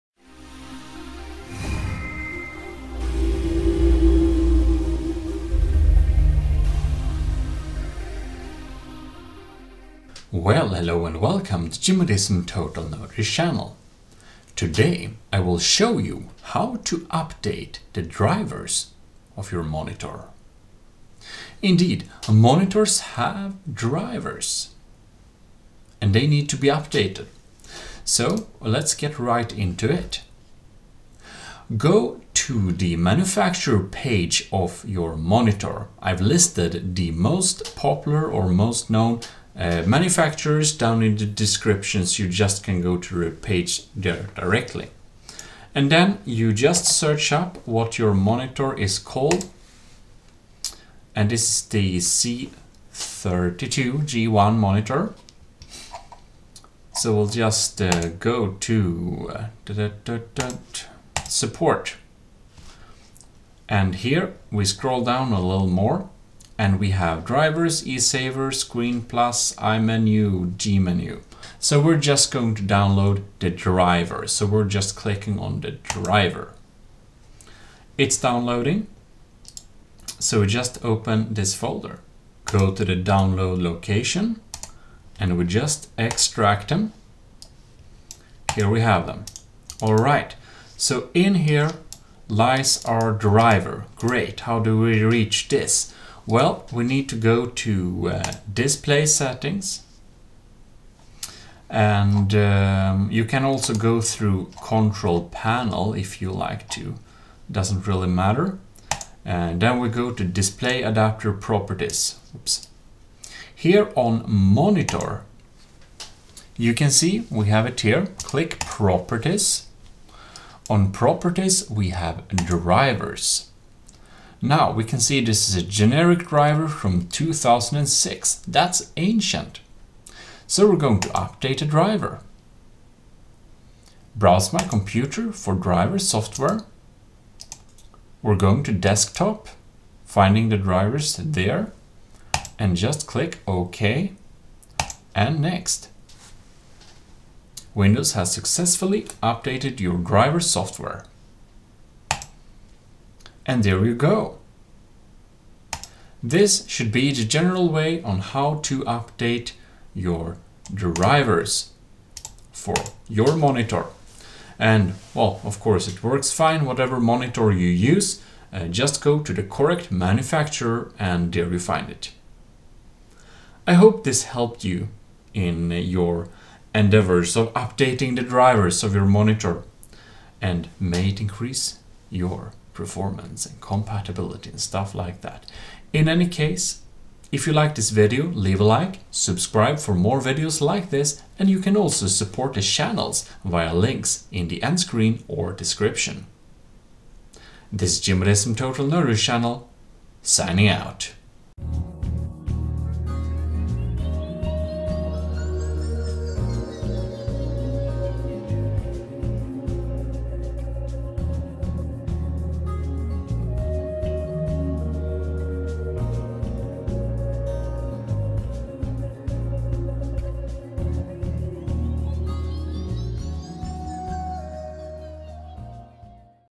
How to Install/Update Drivers for Your Monitor, Screen or Gaming Display - Fix Monitor Driver Issues
This tutorial explains to you how to install drivers for your monitor or display, as it uses Windows default drivers, it's more correct to say that you update your monitor drivers. This should fix most monitor driver issues and compatibility issues with Windows systems.
If you don't install the correct drivers you might cap your screens capabilities, this tutorial will also help if your monitor can't output the FPS it says it should be able to do, if your GPU is not overloaded that is. To know what Hertz you currently run at you can check via this homepage

Top donations are displayed in the hall of fame on my twitch page.

║ CHECK MY OTHER TECH TUTORIALS

Licensed to me according to our agreements.